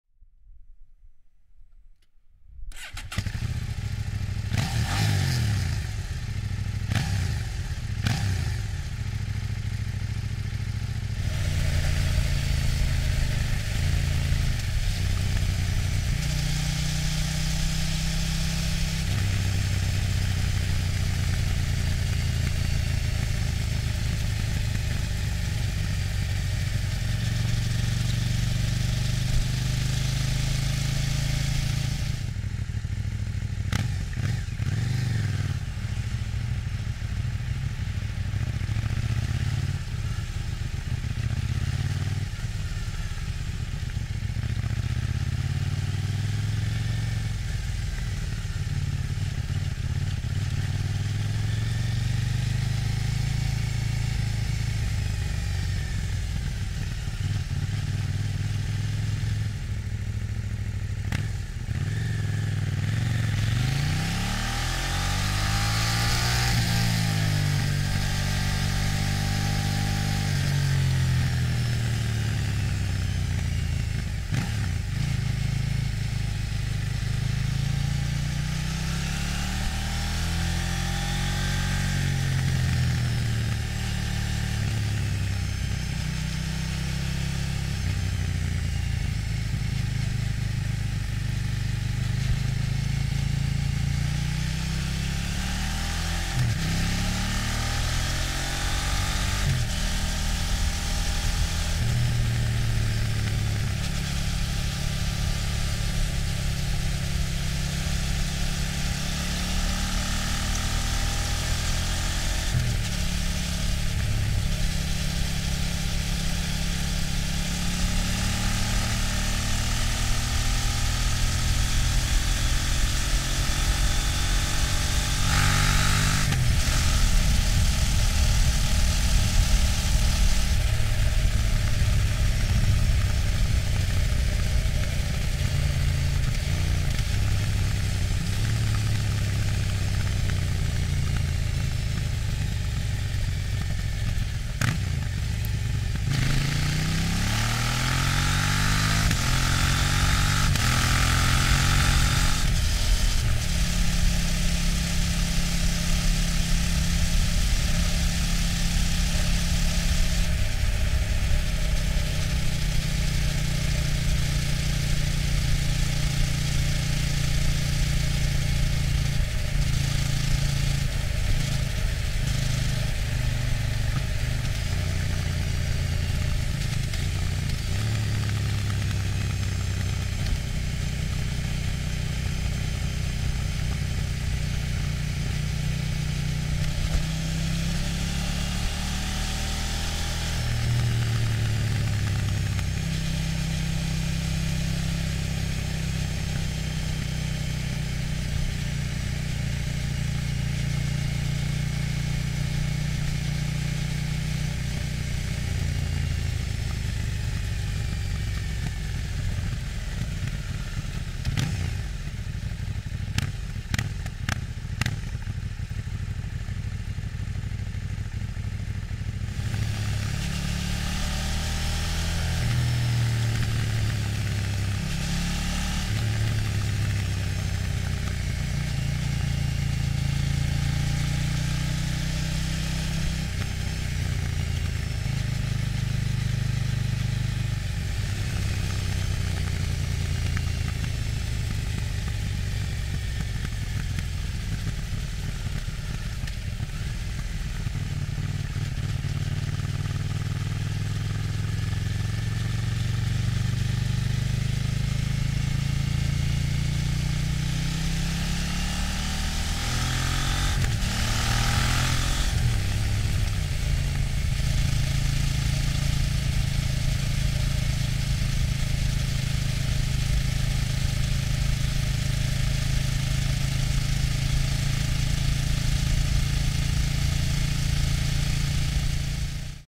Vlog#40 790DUKE Onboard Sound 🎧ヘッドフォン推奨
Vlog#40
790DUKE LC8c RAW-SOUND
Recommended headphones.
🎧dヘッドホン推奨

MOTO:KTM 790DUKE
　　　 LC8c Engine 104.6ps(77Kw) / 8.76kg(86Nm)
　　　 Normal Exhaust

MOTO: KTM 790DUKE
Cam: GoPro HERO7 Black + GoPro HERO6 Black
Rec  : Zoom H5 (USE)